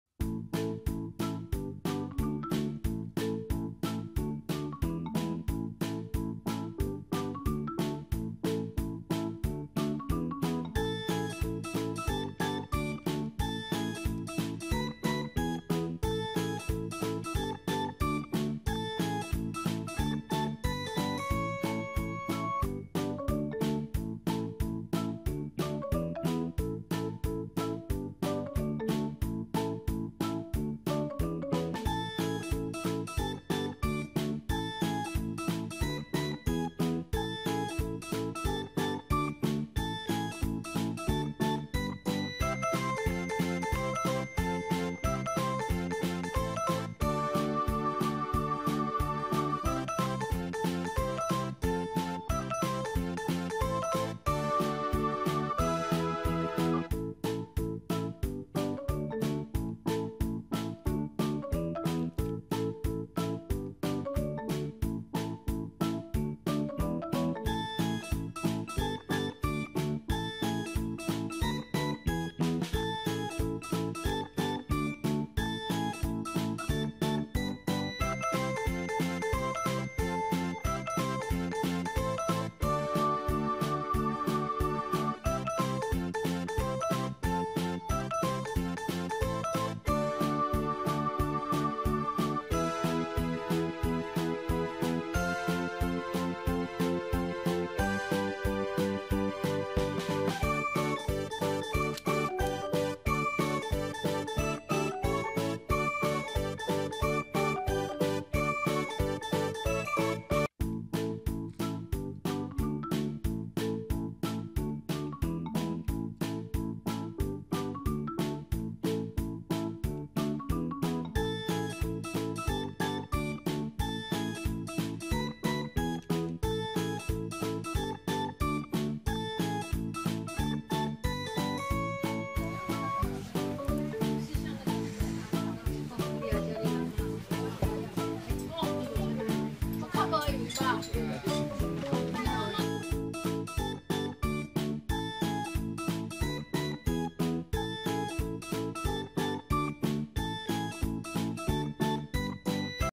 Unboxing TOYS Review/Demos - Tomica Car ford focus metal die cast black silver rims
Grandma toystore brings to you the unboxing demo/review of Tomica car towing truck metal die cast 2 trucks available haul cargo load.  Put together the the towing truck with either a red blue truck to match the tow.